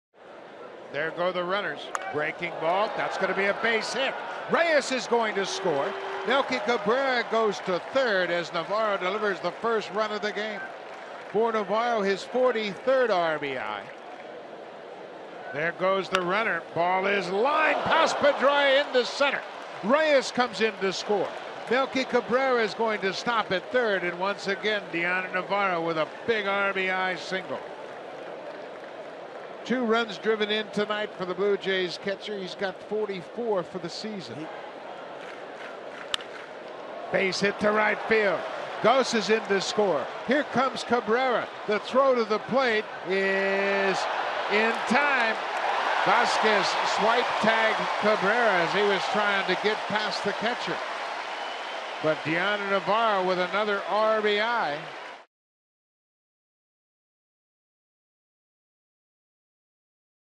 TOR@BOS: Navarro tallies three hits, three RBIs
7/30/14: Dioner Navarro notches three RBI singles, collects three RBIs and a walk in a 6-1 win over the Red Sox